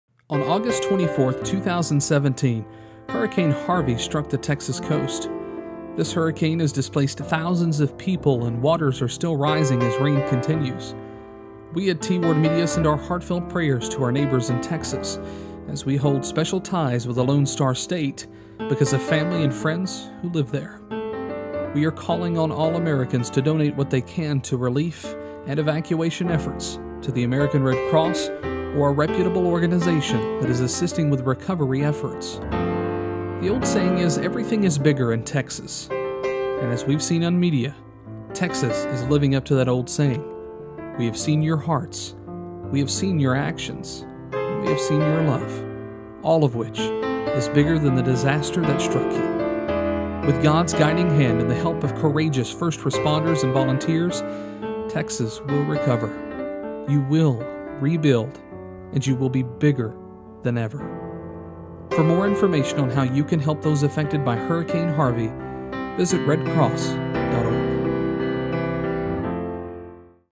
Call to Action for Hurricane Harvey
On August 24, 2017 Hurricane Harvey struck the coast of Texas around Corpus Christi and Rockport. This Hurricane has displaced thousands of people and waters are still rising, as rain continues.

We at T. Ward Media, send our heartfelt prayers to our neighbors in Texas. We hold special ties with Texas, because of family and friends who live there. We are calling on all Americans to donate what they can to relief and evacuation efforts to the American Red Cross or a reputable orginazation that is assisting with recovery efforts.

The old saying is, everything is bigger in Texas. Texas lives up to that old saying. We have seen your hearts. We have seen you actions. We have seen your love. All of which, is bigger than the disaster that has struck you. With God’s guiding hand, and the help of courages first responders and volunteers, you will recover. You will rebuild, and you will be bigger than ever.

For more information on how you can help those affected by Hurrican Harvey, visit RedCross.org.